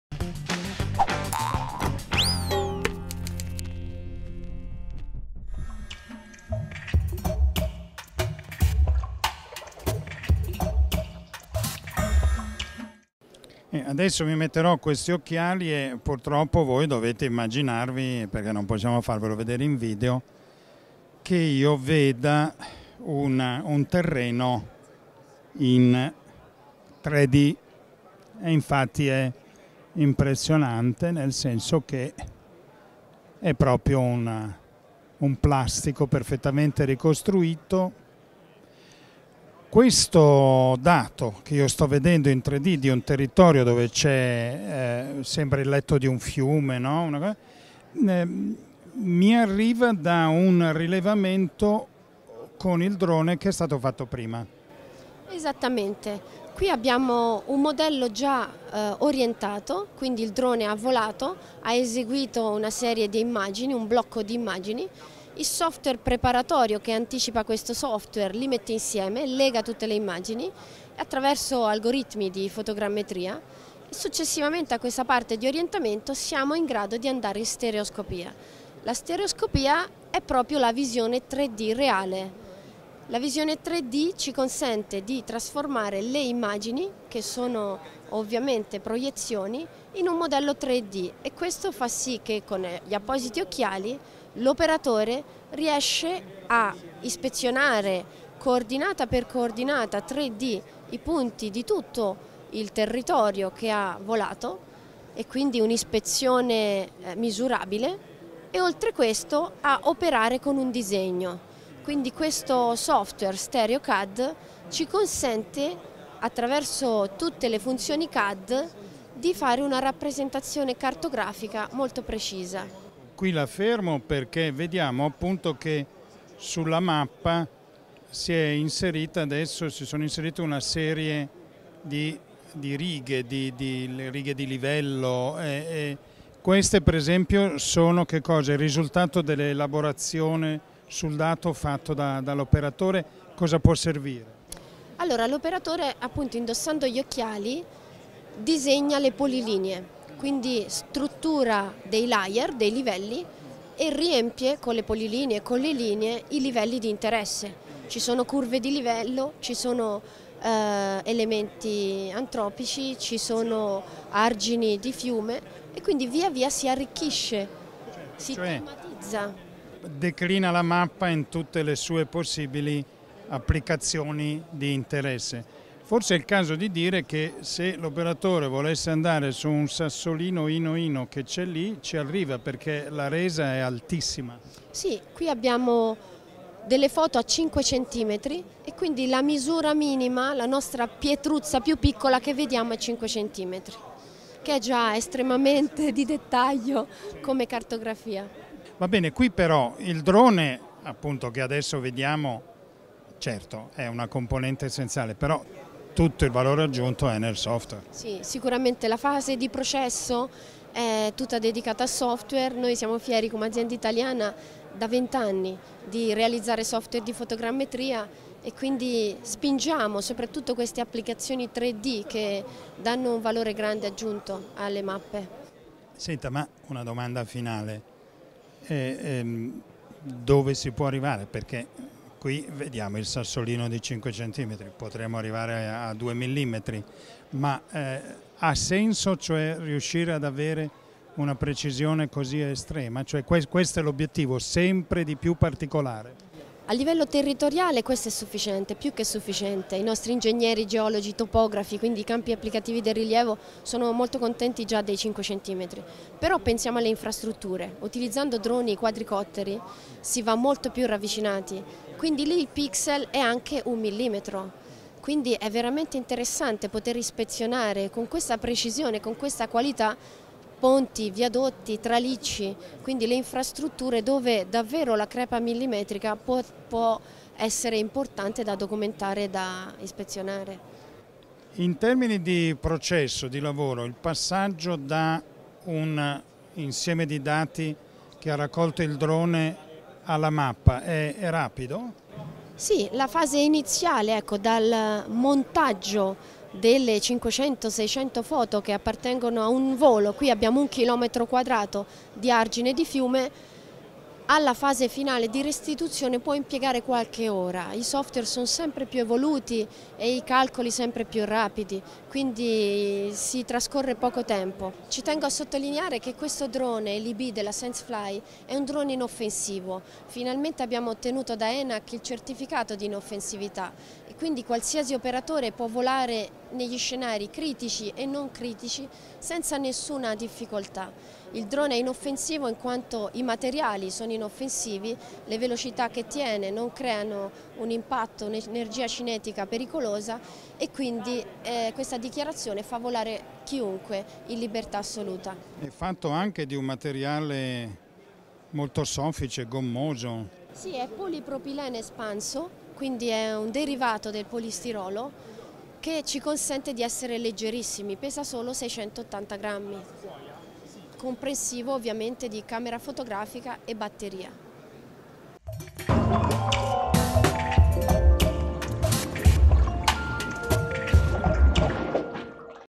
Dronitaly 2016 - StereoCAD - video TRIWU by Federico Pedrocchi
Francesca Ceccaroni presents StereoCAD.
StereoCAD is an innovative software designed to make smart and modern the use of stereoscopic images from drone. With StereoCAD you can perform extremely accurate inspections and 3D measurements, thanks to active stereoscopic perception. At the same time, there are advanced CAD commands ideal for cartographic and architectural plotting. StereoCAD returns you to the real 3D world!